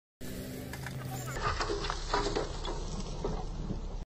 Kubota Mini Excavator Loading a Tractor Trolley in India
Top mini excavator brands.